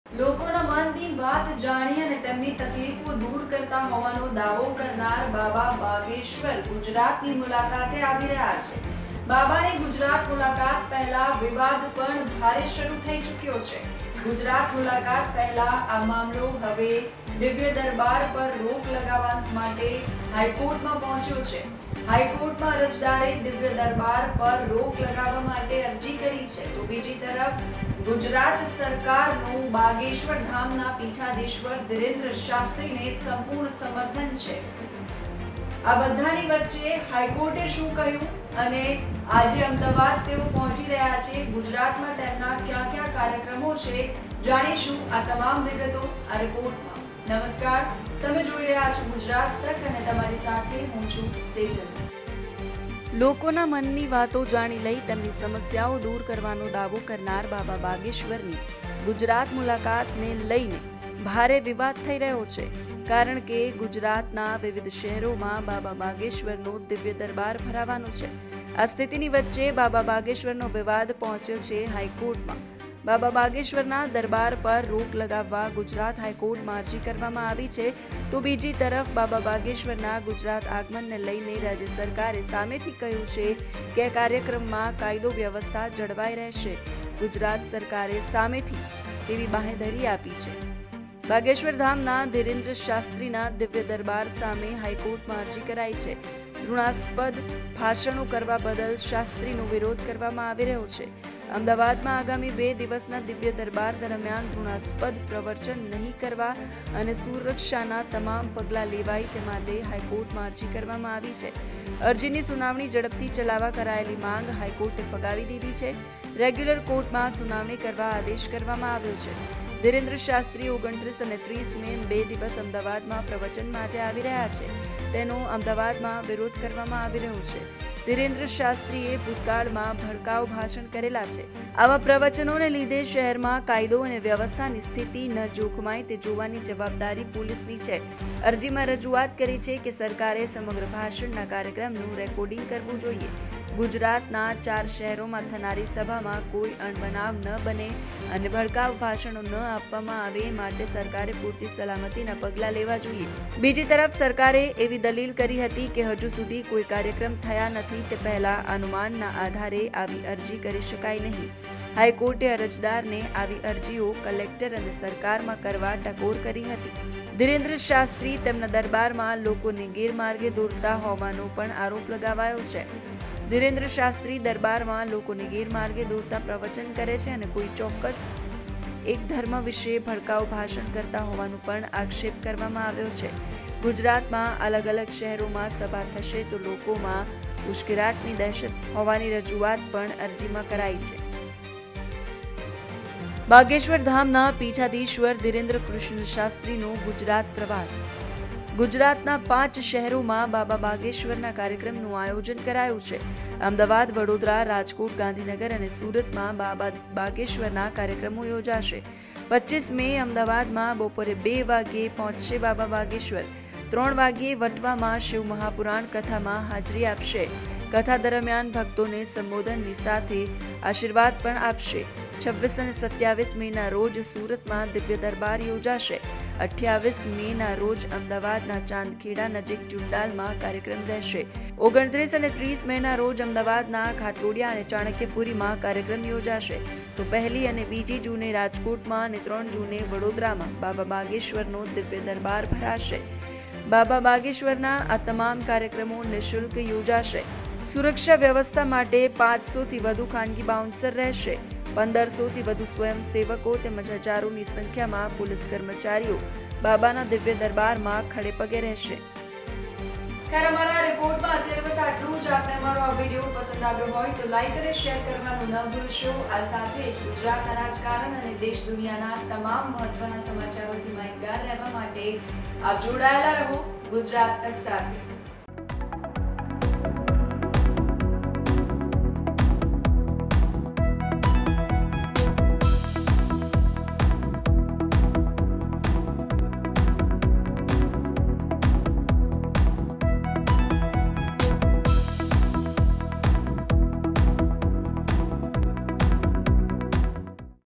Baba Bageshwar Gujarat Visit: High Court માં Bageshwar નો વિવાદ, બાબા આજે Ahemedabad માં...! | GT
મનની વાતો જાણી લેતા હોવાનો અને લોકોની તમામ તકલીફો દૂર કરતા હોવાનો દાવો કરનારા બાબા બાગેશ્વરની ગુજરાત મુલાકાતને લઈને ભારે વિવાદ થઈ રહ્યો છે. કારણકે, ગુજરાતના વિવિધ શહેરોમાં બાબા બાગેશ્વરનો દિવ્ય દરબાર ભરાવાનો છે. આ સ્થિતિની વચ્ચે બાબા બાગેશ્વરનો વિવાદ પહોંચ્યો હાઇકોર્ટમાં. બાબા બાગેશ્વરના દરબાર પર રોક લગાવવા ગુજરાત હાઈકોર્ટમાં અરજી કરવામાં આવી છે. તો બીજી તરફ બાબા બાગેશ્વરના ગુજરાત આગમનને લઈને ગુજરાત સરકારનું પૂર્ણ સમર્થન છે. આ બધાની વચ્ચે હાઈકોર્ટે શું કહ્યું. સાથે આજે બાબા બાગેશ્વરનો અમદાવાદમાં દિવ્ય દરબાર છે.. શું છે વિગતો જાણીશું આ રિપોર્ટમાં...